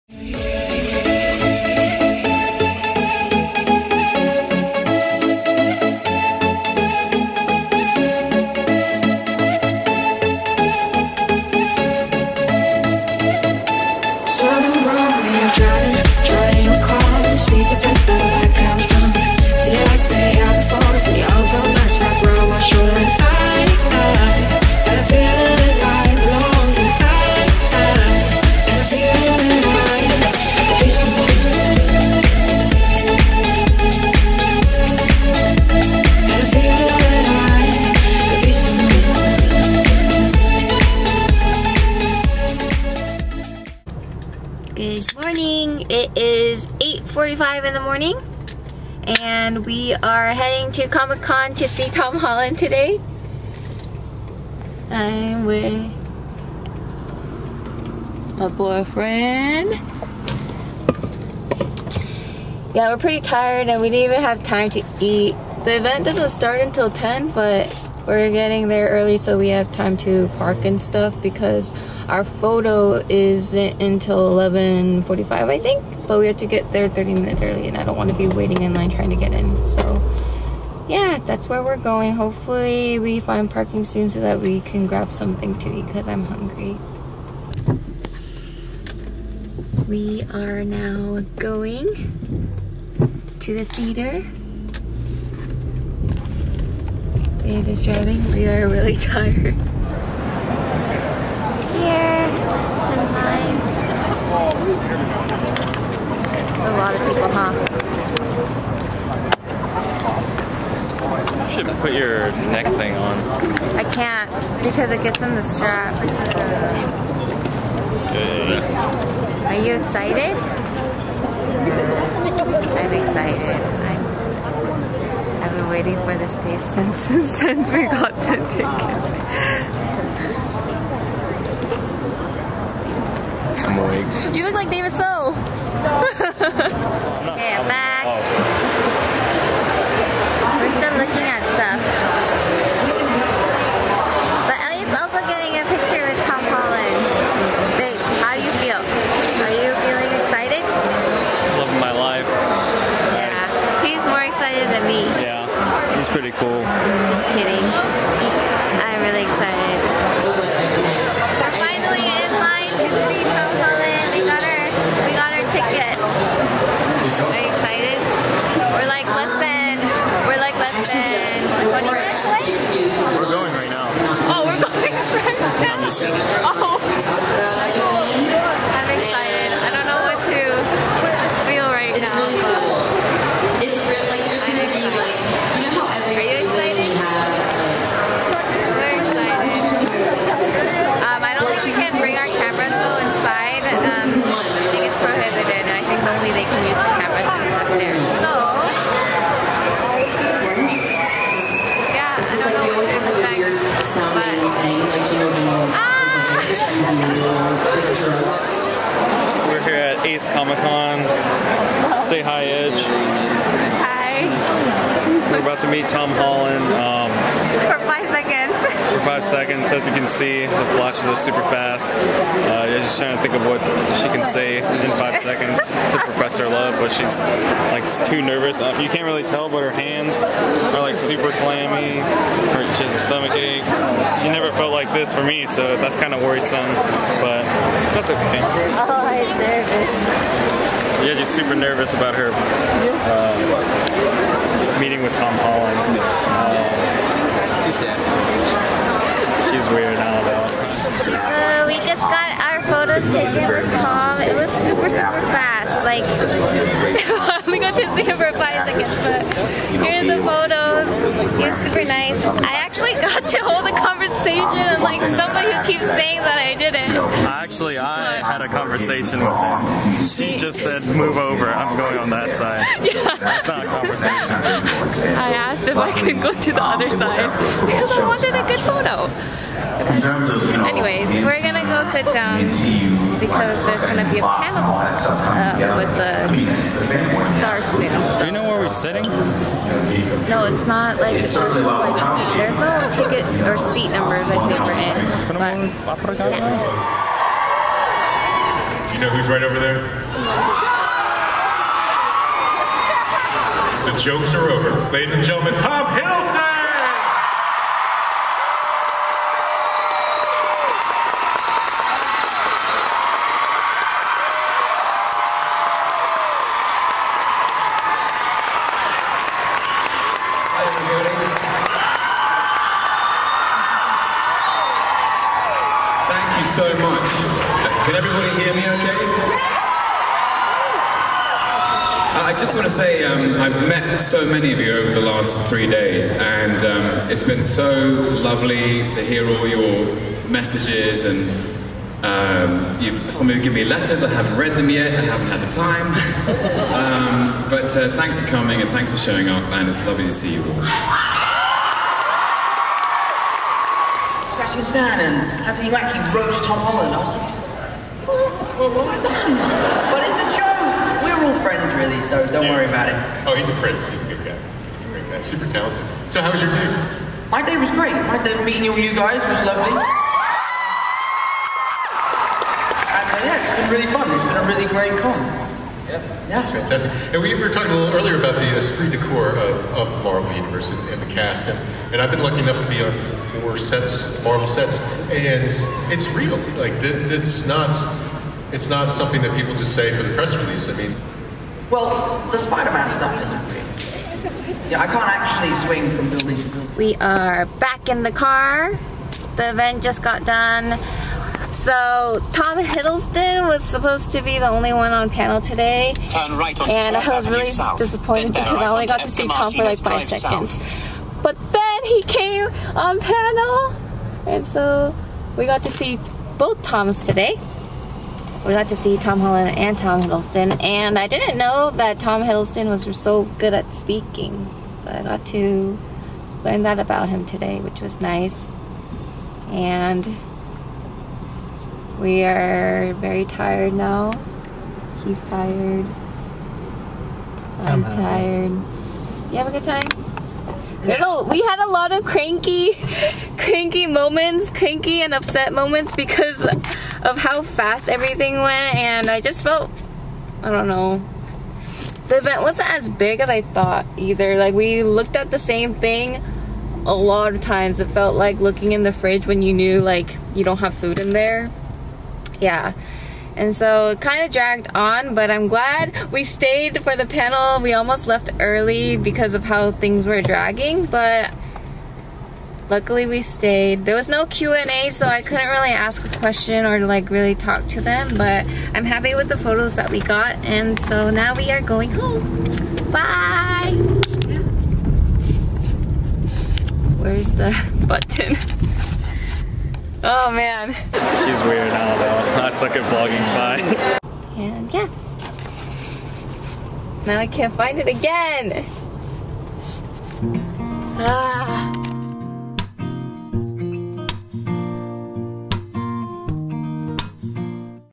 First Vlog: Tom Holland
Hi everyone!
This is my very very first vlog and YouTube video!
There is a lot to improve, but I'm so excited and happy to start something I have always wanted to do.

This vlog is especially special to me not only because I finally gathered the courage to begin a channel, but also because I had the opportunity to meet someone dear to my heart.

Thank you to my brother for giving me this opportunity
Thank you to my boyfriend for taking me to this event

I hope you enjoyed walking through this experience with me!

_Music_